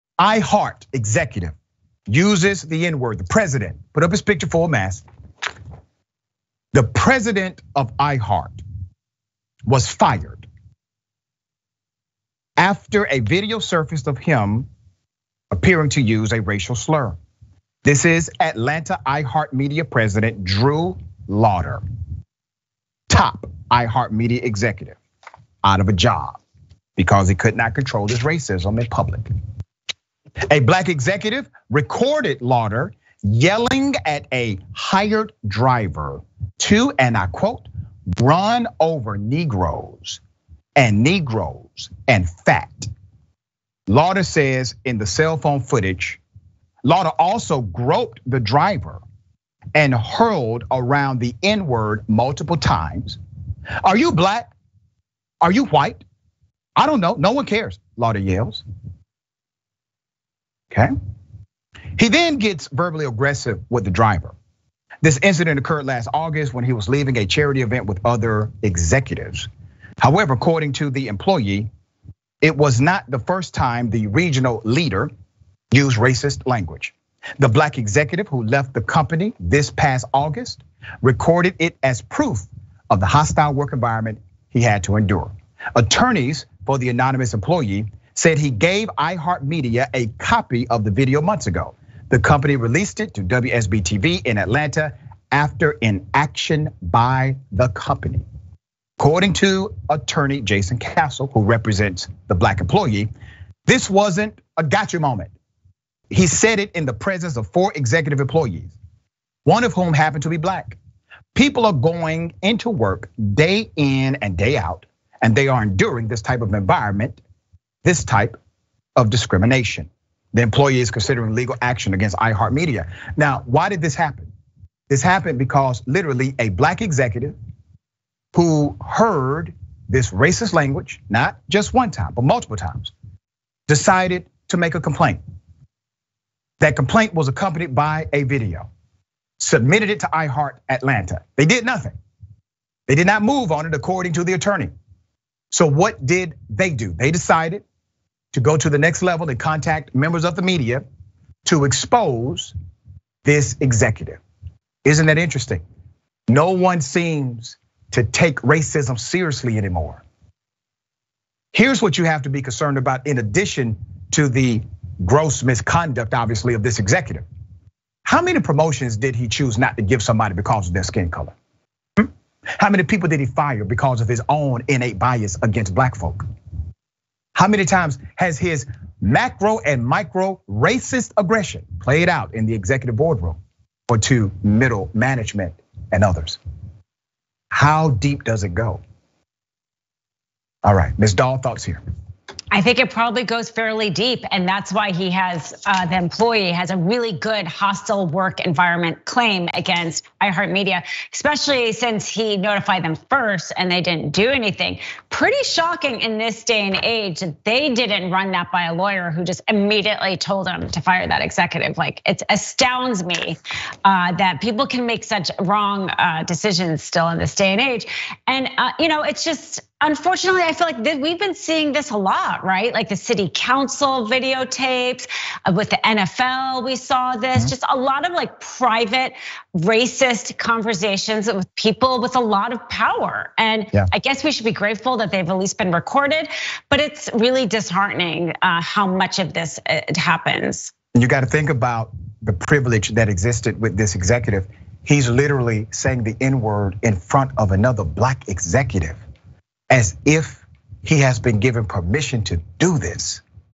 iHeartMedia President Fired For Using Racial Slurs At Driver
iHeartMedia Atlanta president Drew Lauter was recorded by a Black Executive yelling at a driver and using the n-word. Stunningly, the company did not fire Lauter until after the video was made public. Dr. Rashad Richey and Dina Sayegh Doll discuss on Indisputable.

"A top iHeartMedia executive is out of a job after video of him using racist language was made public.

A Black executive recorded iHeartMedia Atlanta president Drew Lauter yelling at a hired driver to “run over Negroes,” and using the N-word while leaving a charity event last August."

221021__ID07ATL